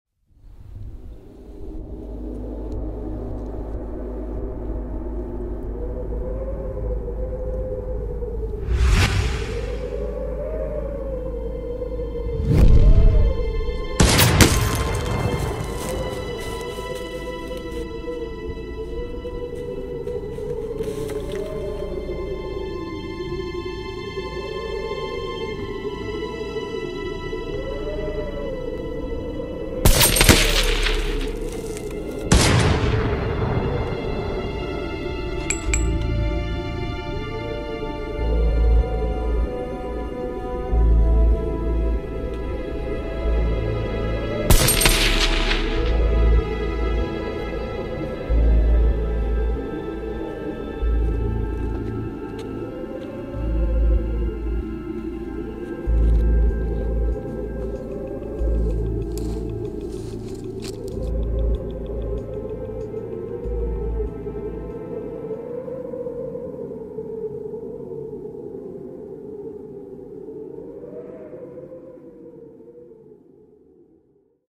SNIPER SHOOTOUT | 4k
Suspenseful sniper shootout short film.